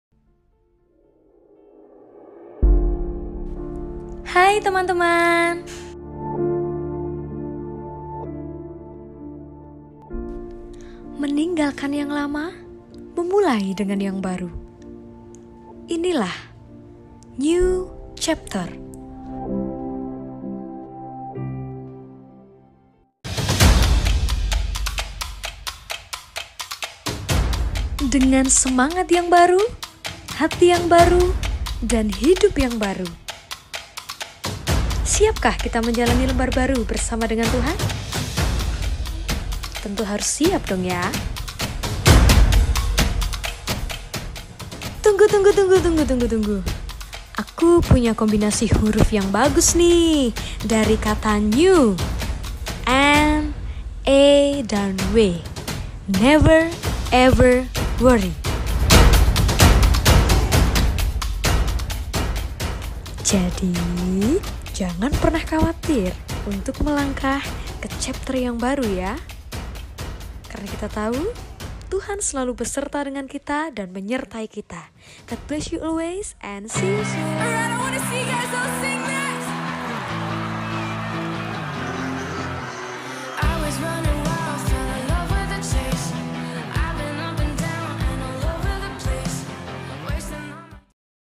Trailer Minggu Anugerah 2021 - Rokris SMA Negeri 3 Surakarta
Shallom teman-teman yang diberkati Tuhan🤗🙌🏻

1 tahun ajaran sekolah telah kita lewati dan setengah tahun 2021 sudah kita lewati dengan penyertaan Tuhan yang tidak pernah lepas dari hidup kita. Masuk ke dalam bab baru dalam kehidupan adalah di mana kita memulai semua dari awal dengan keadaan yang lebih baru untuk menjadi pribadi yang lebih baik

Penyertaan Tuhan akan nyata di fase yang baru dan Tuhan sudah menyediakan berkat-berkat baru bagi kita semua. Roh yang baru untuk memulai fase yang baru. Meninggalkan kehidupan lama yang penuh dosa ke kehidupan yang baru. Memulai hidup yang baru sebagai pelayan Tuhan yang setia. So this is,

*Minggu Anugerah 2021*

*✨NEW CHAPTER✨*

▶️Youtube channel " ROKRIS SMAGA SOLO "

Mari kita bersama berproses menjadi manusia-manusia baru di dalam Tuhan lewat ibadah ini dan merasakan penyertaan Tuhan sungguh nyata akan hidup kita🌻

(c)Multimedia Rokris Smaga 2021